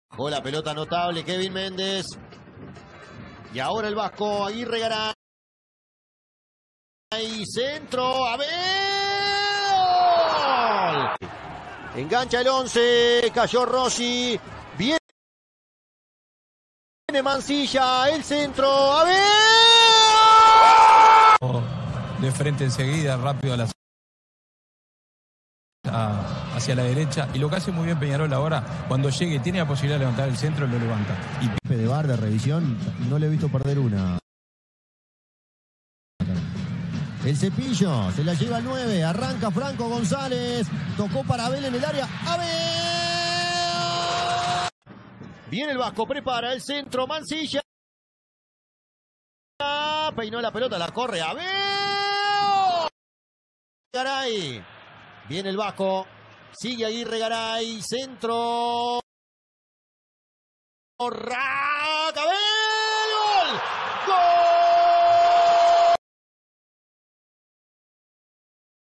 Análisis de rendimiento: Abel Hernandez - Peñarol - Gioca Meglio
Abel Hernández
33 años
Delantero
-12 goles
-219/278 de pases completados. 78,8% de efectividad
-55/130 de duelos aéreos ganados. 42,3% de efectividad
-26/45 balones recuperados en mitad adversaria. 57,8% de efectividad

-Analista de rendimiento: @‌trovatosantiago